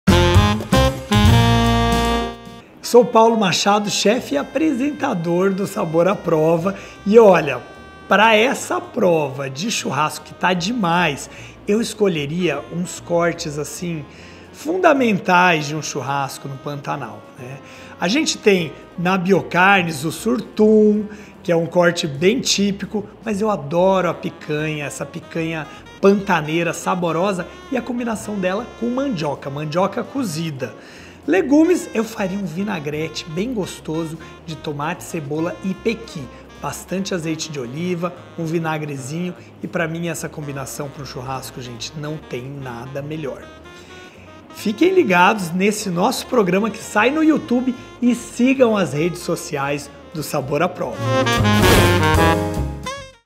Quer saber como seria o churrasco do Chef Paulo Machado?
Vocês também ficaram curiosos para saber como seria um churrasco do nosso Chef Paulo Machado ??

Dê play no vídeo e fique com água na boca com a gente.

Chef Paulo Machado a produção do reality está aguardando esse churrasco eim🥩🔥